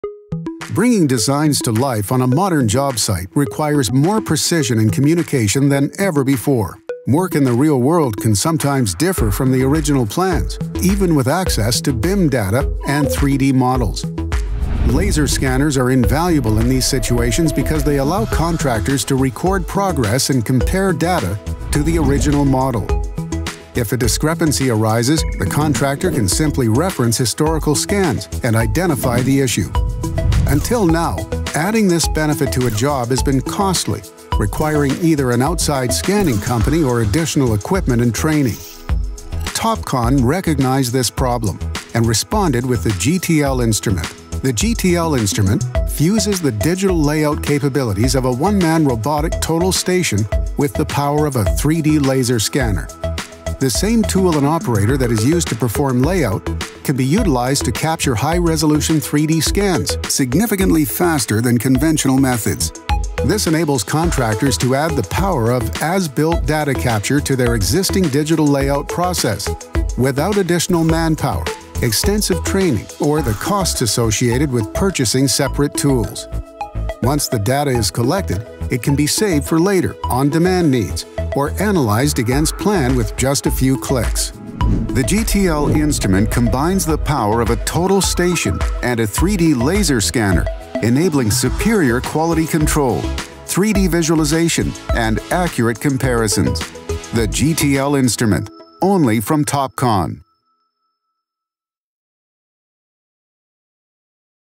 GTL Series — Robotic Scanner Explained
The GTL Series combines the power of a robotic total station with a best-in-class laser scanner to perform digital layout and capture high-res 3D scans in a single setup. Simplify the scanning process and dramatically reduce the need for post-processing by capturing scan data on survey control. The workflow seamlessly integrates with ClearEdge3D software for advanced construction QA workflows and as-builts.